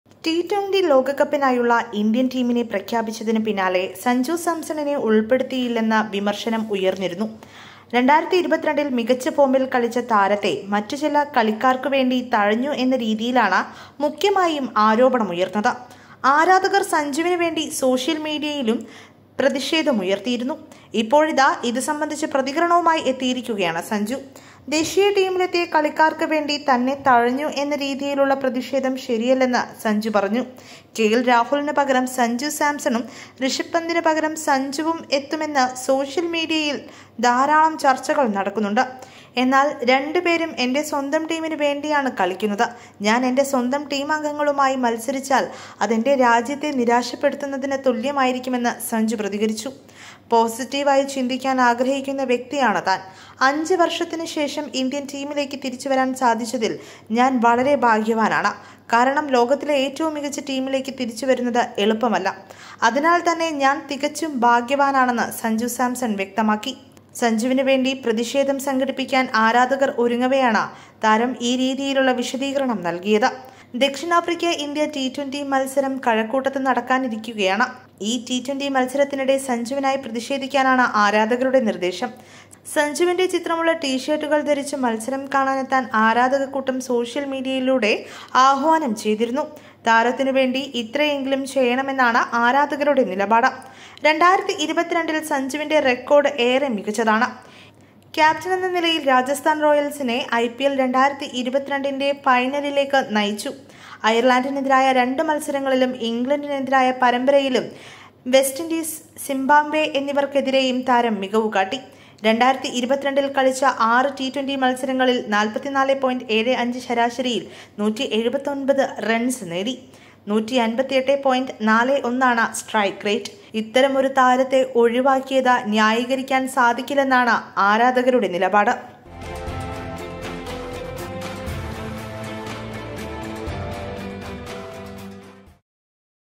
തനിക്കുവേണ്ടിയുള്ള പ്രതിഷേധങ്ങൾ ഒഴിവാക്കണമെന്ന സൂചനയുമായി സഞ്ജു|Sanju samson
ടി20 ലോകകപ്പിനായുള്ള ഇന്ത്യൻ ടീമിനെ പ്രഖ്യാപിച്ചതിന് പിന്നാലെ സഞ്ജു സാംസണിനെ ഉൾപ്പെടുത്തിയില്ലെന്ന് വിമർശനം ഉയർന്നിരുന്നു. 2022ൽ മികച്ച ഫോമിൽ കളിച്ച താരത്തെ മറ്റു ചില കളിക്കാർക്കുവേണ്ടി തഴഞ്ഞു എന്ന രീതിയിലാണ് മുഖ്യമായും ആരോപണം ഉയർന്നത്. ആരാധകർ സഞ്ജുവിന് വേണ്ടി സോഷ്യൽ മീഡിയയിലും പ്രതിഷേധവുമായെത്തി. ഇപ്പോഴിതാ ഇതു സംബന്ധിച്ച് പ്രതികരണവുമായി എത്തിയിരിക്കുകയാണ് സഞ്ജു. ദേശീയ ടീമിലെത്തിയ കളിക്കാർക്ക് വേണ്ടി തന്നെ തഴഞ്ഞു എന്ന രീതിയിലുള്ള പ്രതിഷേധം ശരിയല്ലെന്ന് സഞ്ജു പറഞ്ഞു.